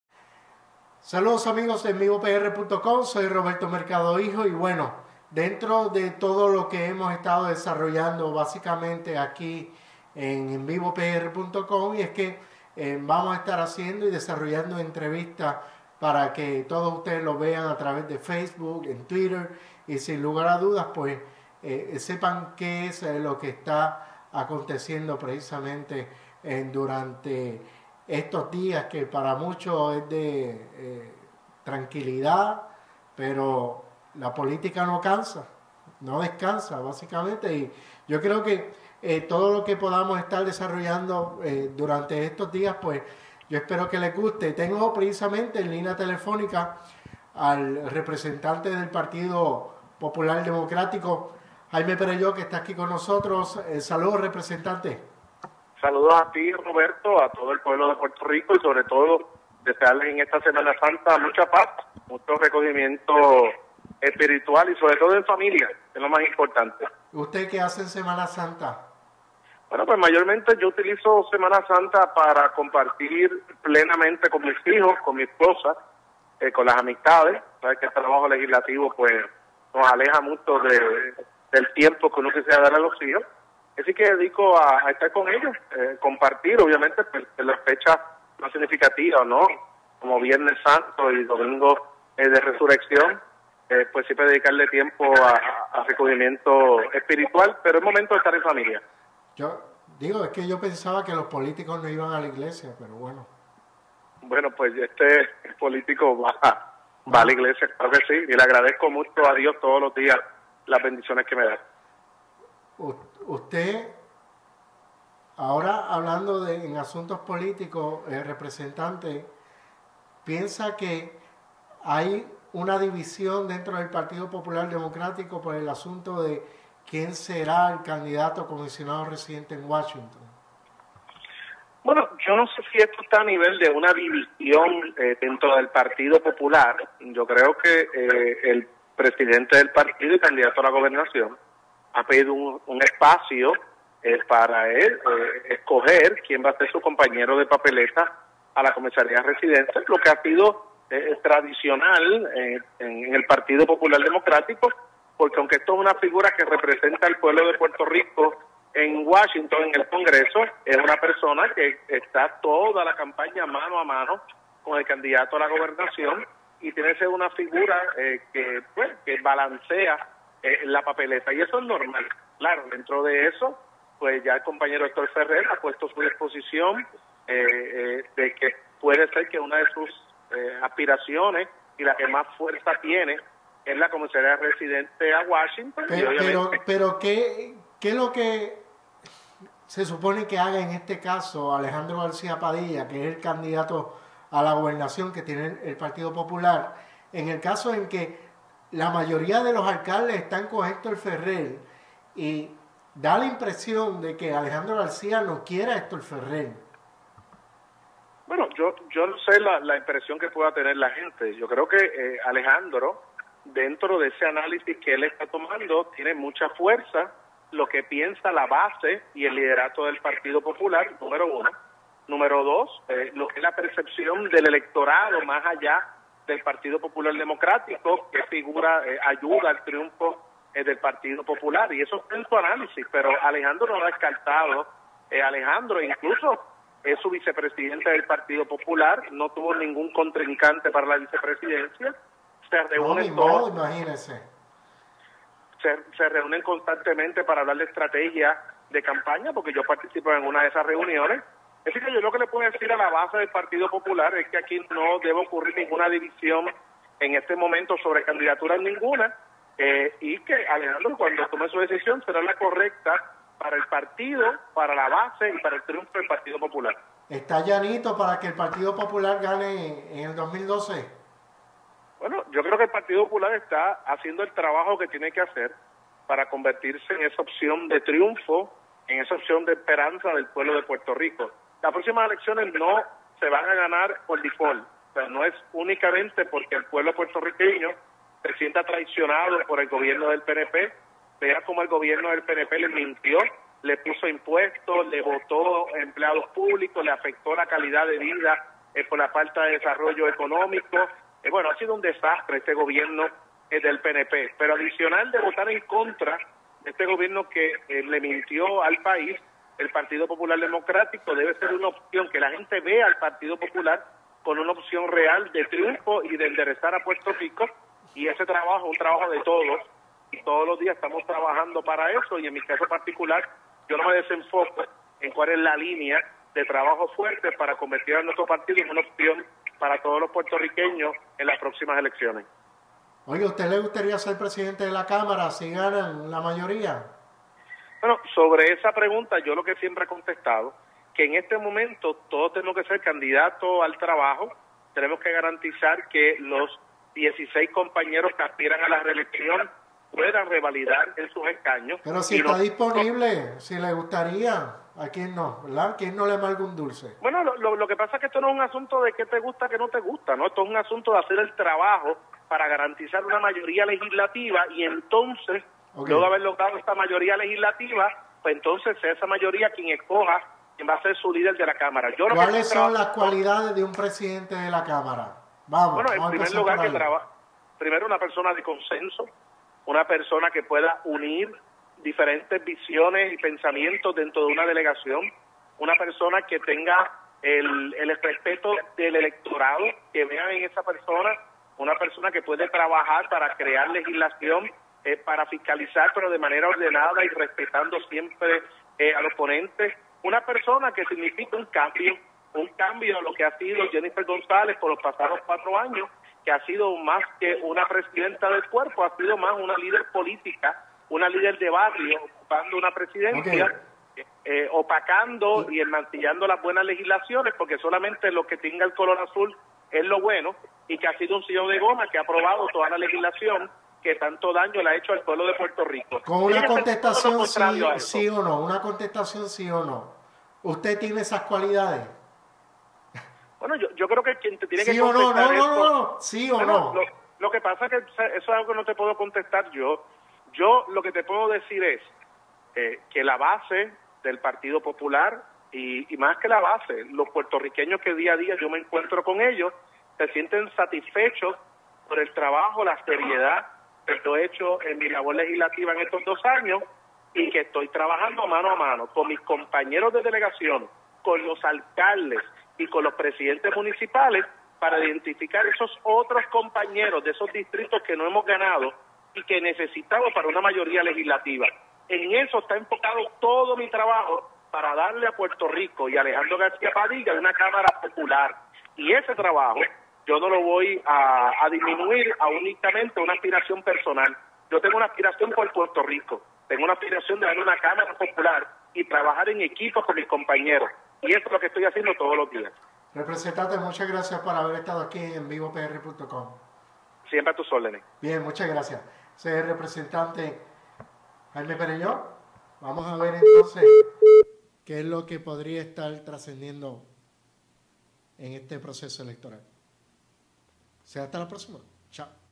Entrevista a Jaime Perelló
Que pasa con Hector Ferrer y Alejandro Garcia Padilla